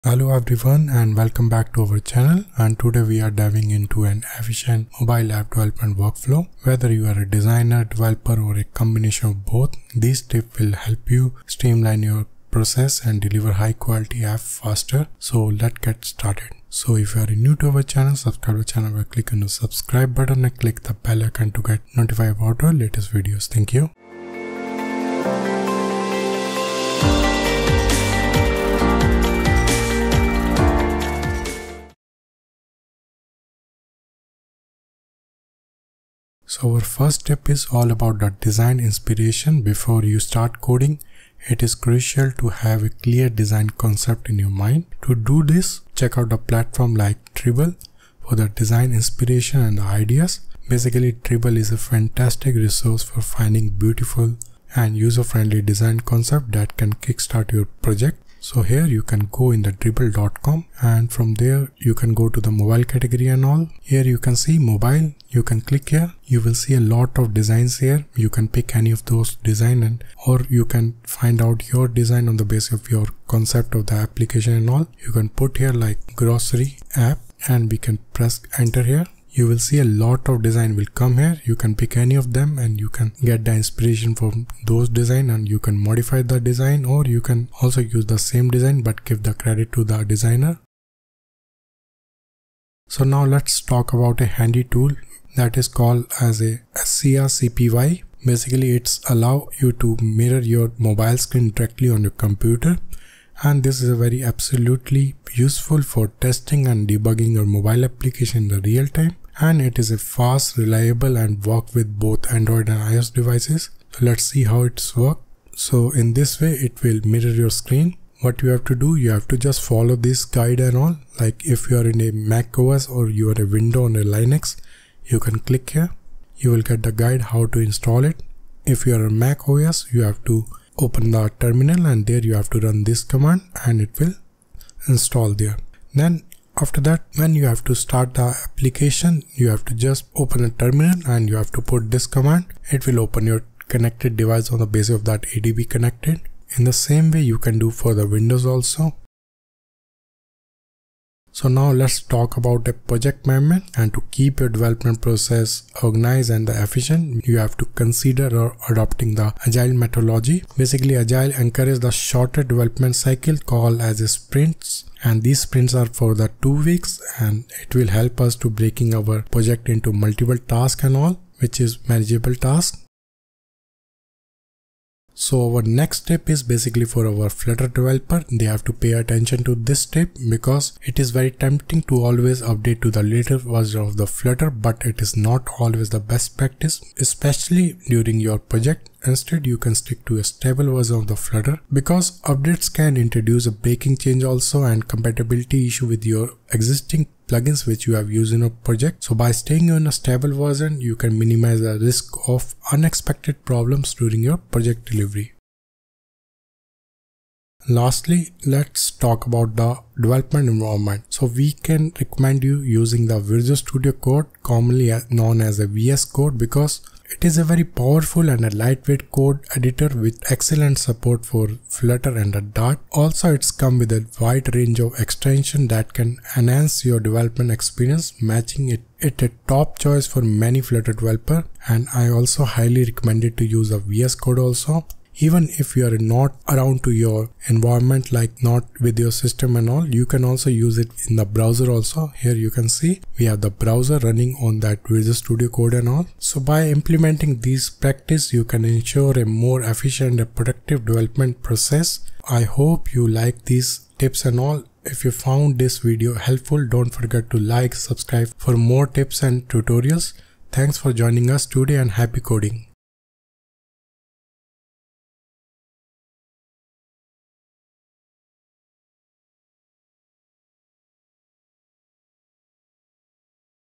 Efficient Mobile App Development Workflow
In this video we're diving into an efficient mobile app development workflow. Whether you're a designer, developer, or a combination of both, these tips will help you streamline your process and deliver high-quality apps faster.

⏱TIMESTAMPS
00:00 - Introduction
00:34 - Check Dribble for Design Concept
01:30 - Use Scrcpy to Mirror Mobile Screen
02:29 - Adopt Agile Methodology with 2-Week Sprints
02:52 - Stick to a Stable Version of Flutter
03:25 - Utilize Visual Studio Code (VSCode)

📄Source Code
Follow us to get Source @ Twitter | Facebook | Instagram | Telegram

📎Packages Used

⛄If you like my work , you can support me
☑️DM For UPI Number